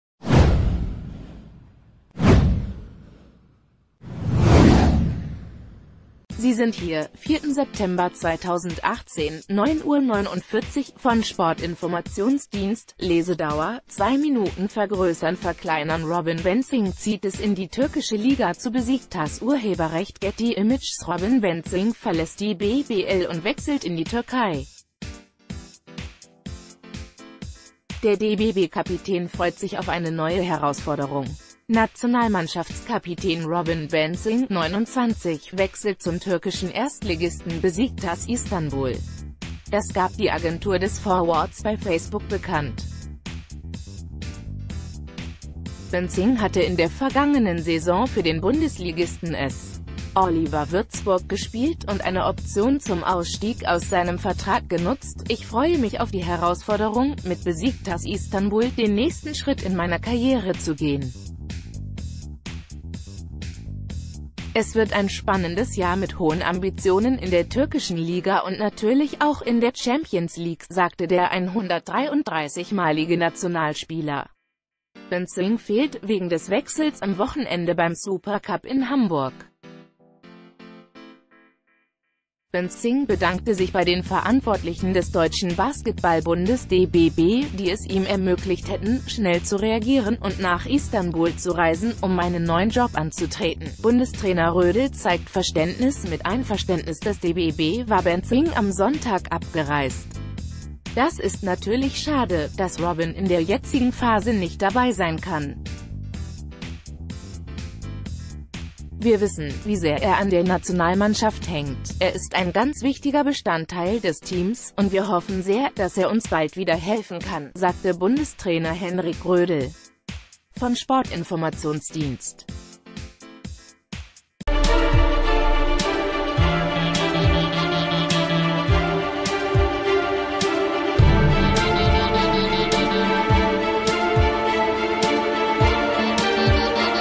Basketball: DBB-Kapitän Robin Benzing wechselt zu Besiktas Istanbul
Sie sind hier: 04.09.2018 / 09:49 Uhr - von Sportinformationsdienst / Lesedauer: 2 Minuten vergrößernverkleinern Robin Benzing zieht es in die türkische Liga zu Besiktas © Getty Images Robin Benzing verlässt die BBL und wechselt in die Türkei. Der DBB-Kapitän freut sich auf eine neue Herausforderung. Nationalmannschaftskapitän Robin Benzing (29) wechselt zum türkischen Erstligisten Besiktas Istanbul. Das gab die Agentur des Forwards bei Facebook bekannt. Benzing hatte in der vergangenen Saison f...